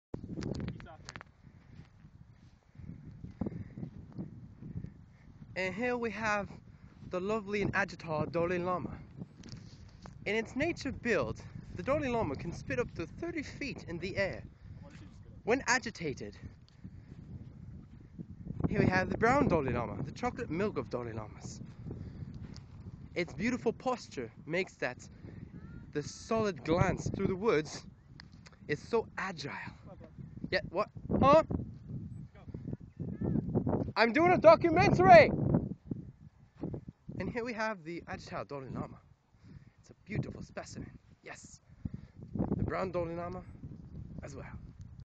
Llama documentary
This is me being a wildlife researcher doin a documentary on the endangered LLAMA !